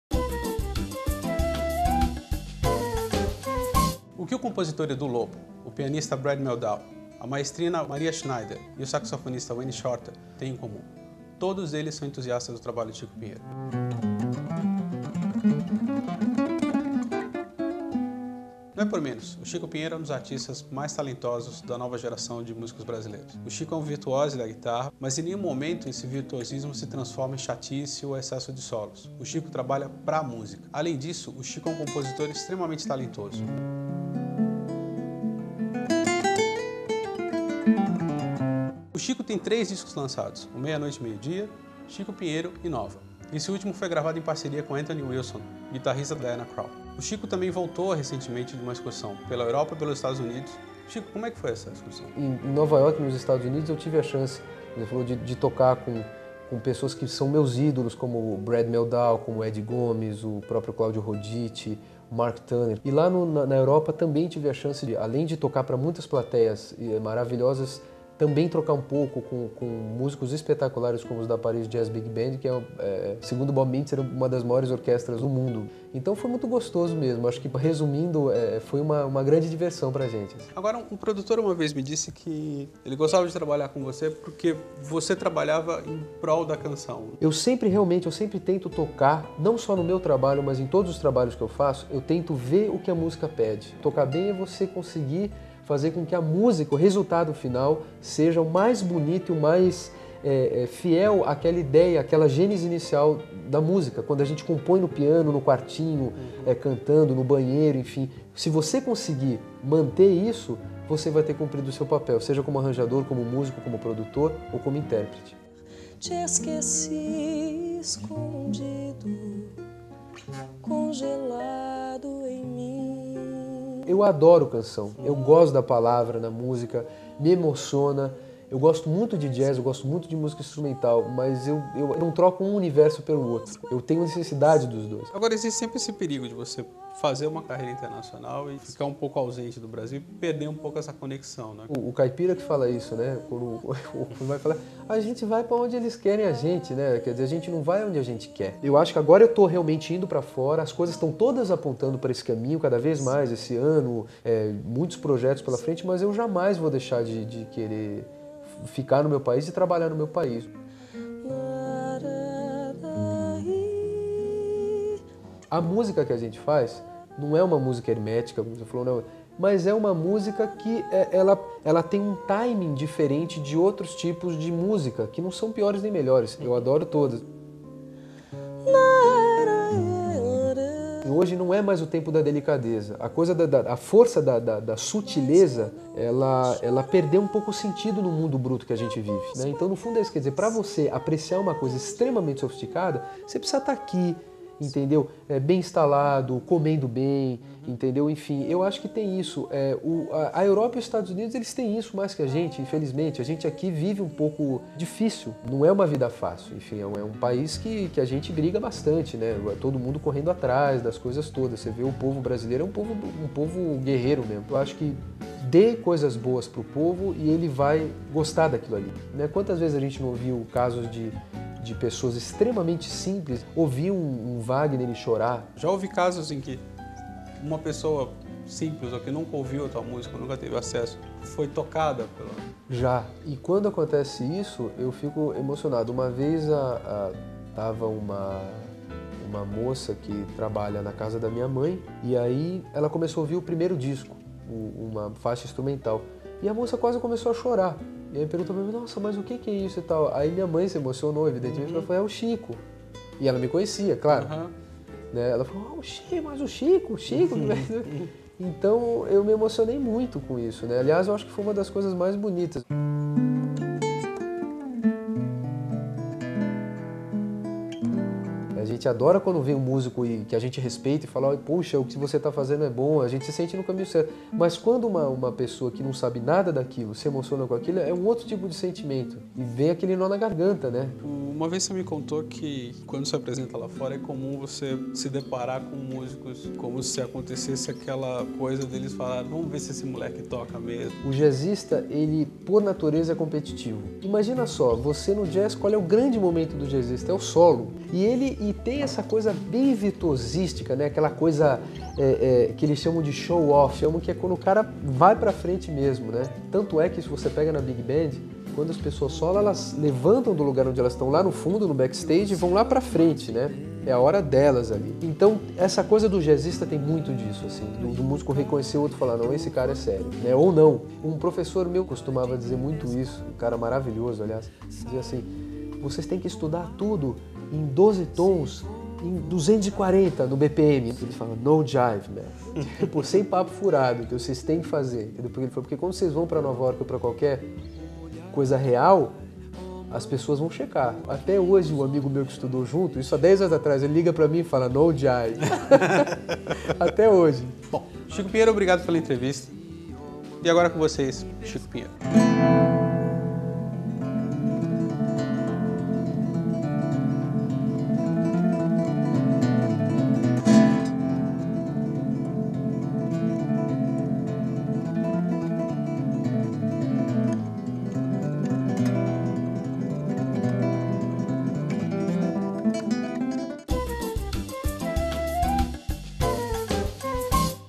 Entrevista com Chico Pinheiro
Acompanhe a entrevista com o violonista, guitarrista e compositor Chico Pinheiro